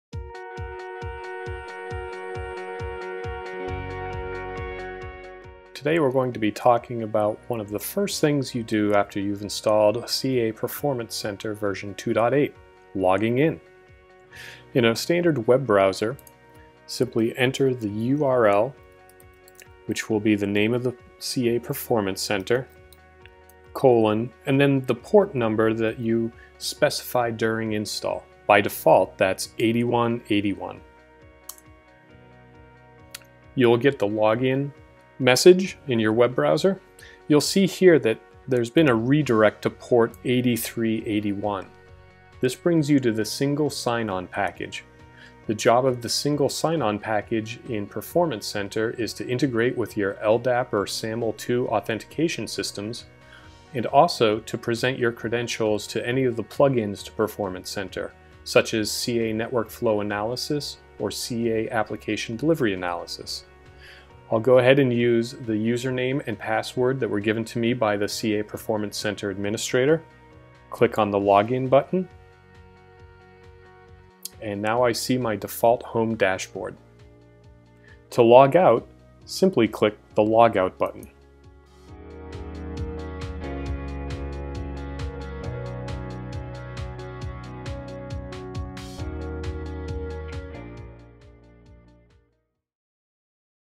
How to login to CA Performance Management
CA Performance Management “How To” video series:  A step-by-step walk through on how to login to CA Performance Management after first-time installation.